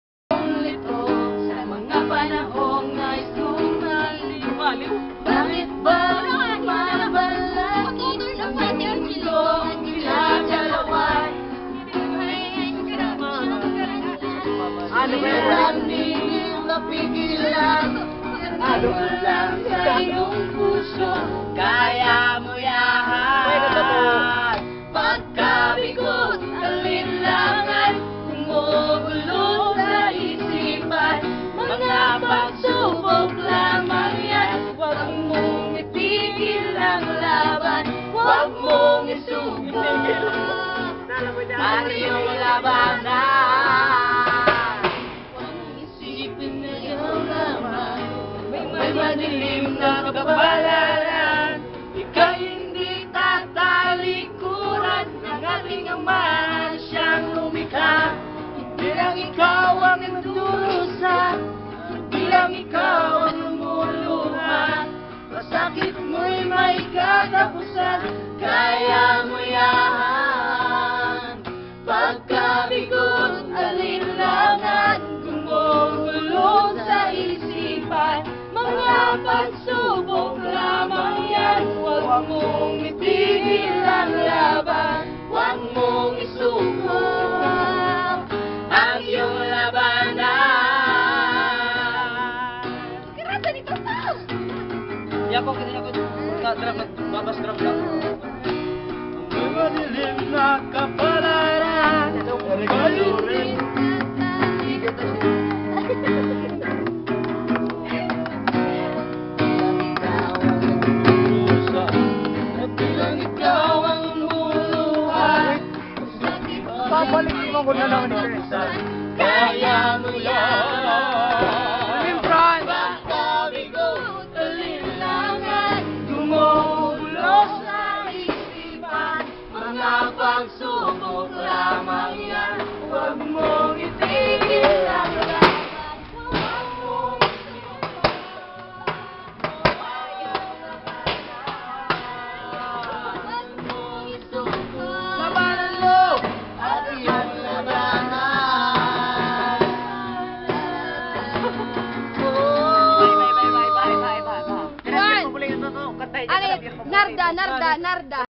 Kantahan sa Magtaon National High School
This Video was taken last March 10, 2015,,,, this is my personal video with my friends inside the campus of Magtaon National High School (MNHS)... On that day we were talking about what course we will take after High school... and it goes to singing. here just watch this guys. :)